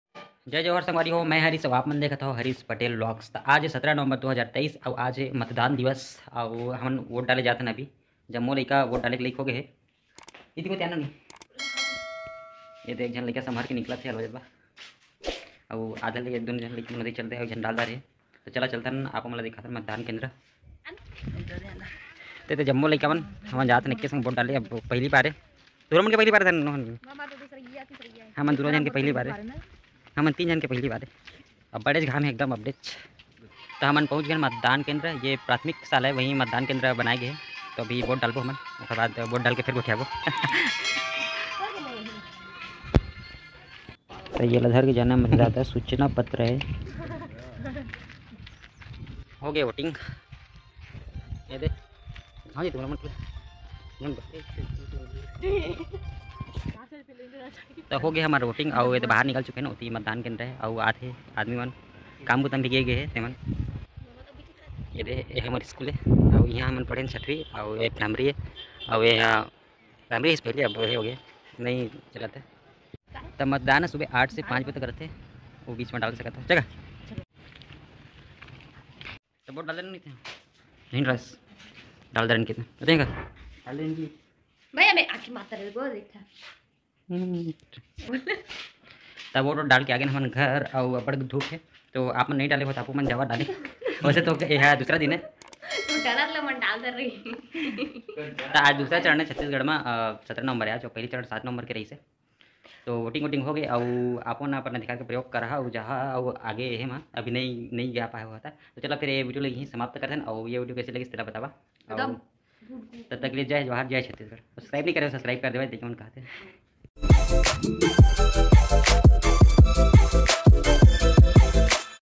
Vote dal Daren | 17 November 2023 | harish patel vlogs
Jai joahar sangawari ho mai hw harish aur aapmn dekhat hw harish patel vlogs.

Ta video me aapman la voting dekhe br milhi hman dale jan han aur hamar prathamik shalaa la matdaan Kendra banaye gehe

Asha kartha aapman la viDeo pasand aahi.

Use Equipments :-

1. Mobile

one plus N10

2. mini tripod -